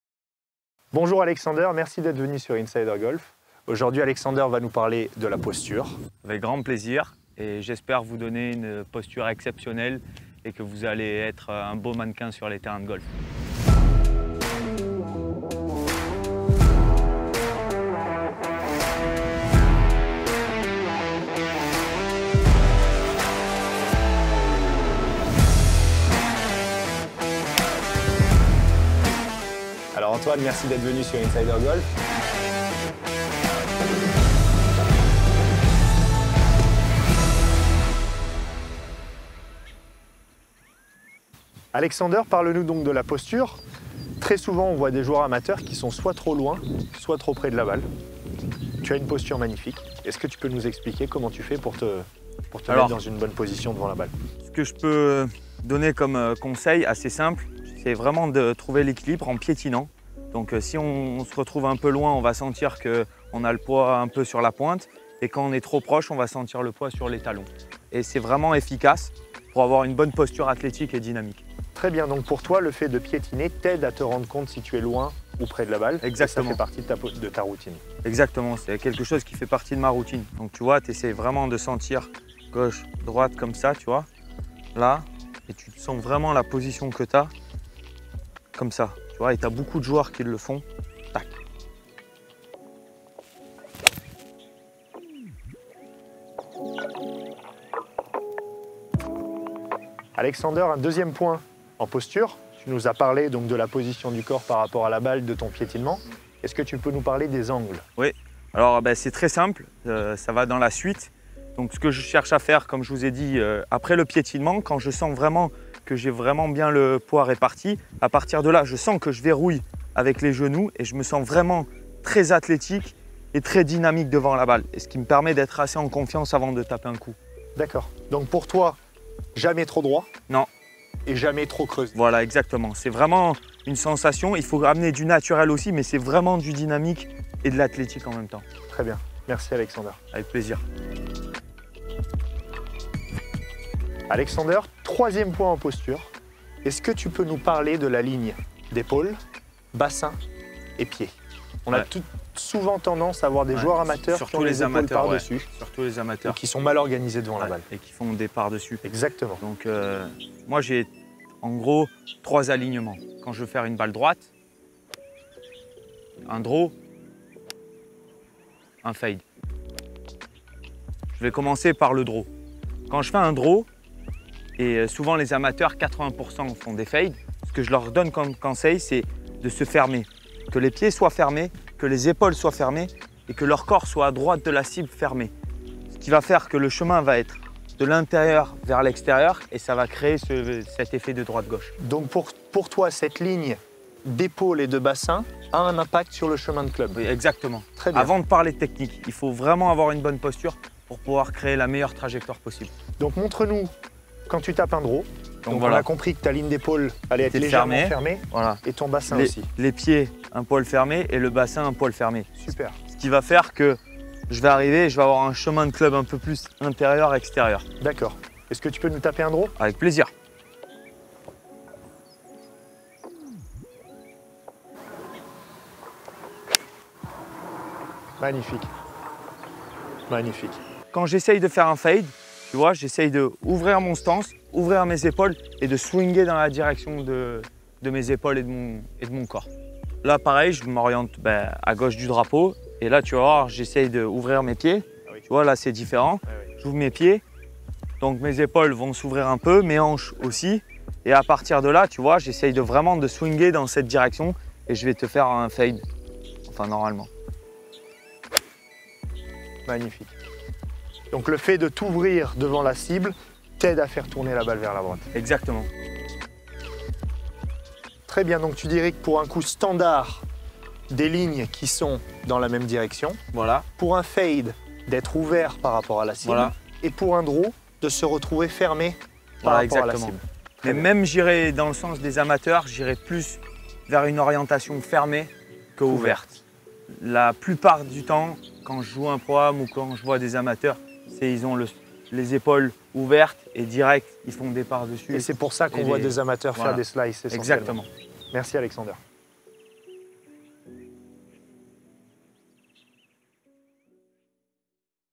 Episode 1 - La Posture (avec Alexander Levy)
L'épisode 1 de Insider Golf vous donne accès à des conseils simples et efficaces de la part d'Alexander Levy. Alors écoutez bien et progressez vous aussi en posture grâce à Insider Golf.

The first episode of Insider golf gives you access to simple and efficient tips from Alexander Levy. Pay attention and improve your posture game with the help of Insider Golf!

Video by @Frames.insta
Player @alexlevy83